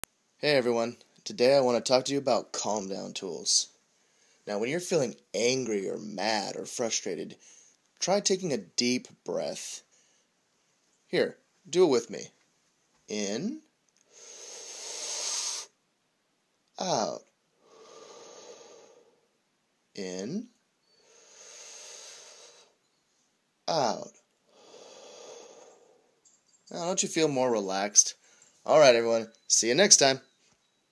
Calm Down Tools: Deep Breath
A short description of the most basic and well known calm down tool.

I've been toying around with using animations to help teach my clients and students with. Obviously this is just a test animation, but any feed back from others, particularly those working with special needs youth, would be appreciated.

*Update* So far these have gone over well.